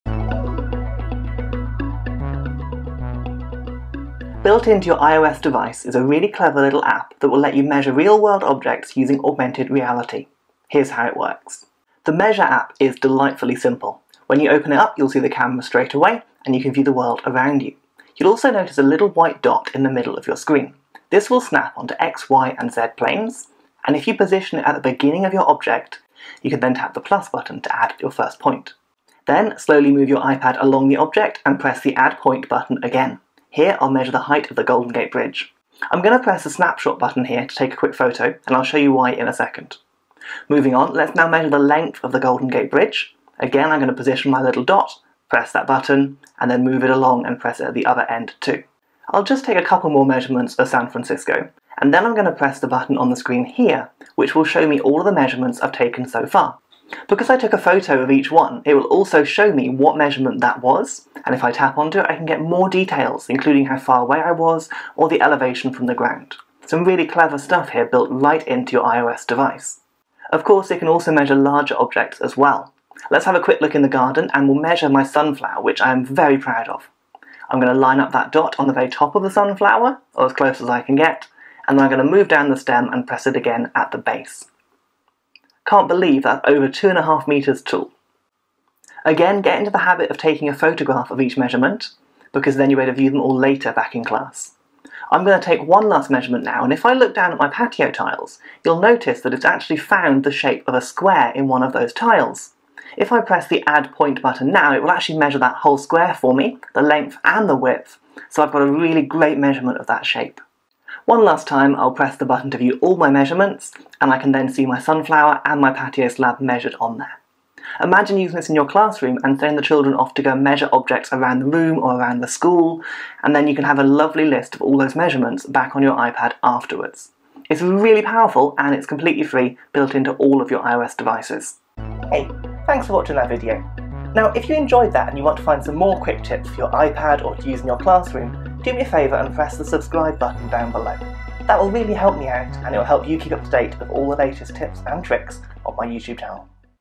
How to Record Measurements on your iPad using the Measure App
This week I'll show you how to master the Measure app that comes as standard on iPad and iPhones, which will let you measure objects inn the real world using Augmented Reality.

By following this tutorial you'll learn how to quickly and accurately measure length, height and area of objects as well as how to take a photo of each measurement so you can refer to them later.  The Measure app, despite being very simple, has a huge potential in the classroom to enable your students to quickly measure anything they need to.  Imagine working out the height of a tree using trigonometry, then confirming by using your iPad Measure app.  Or estimating how far away an object is only to then use the iPad to check afterwards.  It's great!